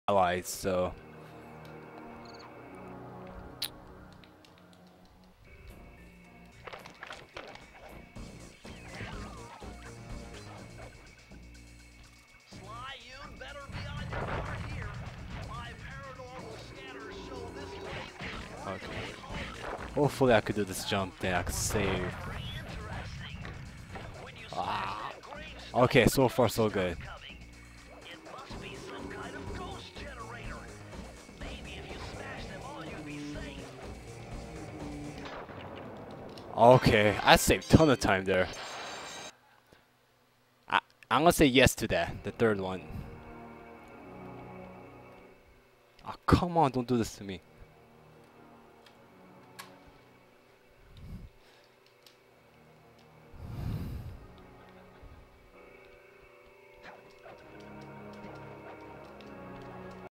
Faster Strat for Grave Undertaking
First time attempt during a run. I call it a success. Even my PS2 couldn't handle the successfulness.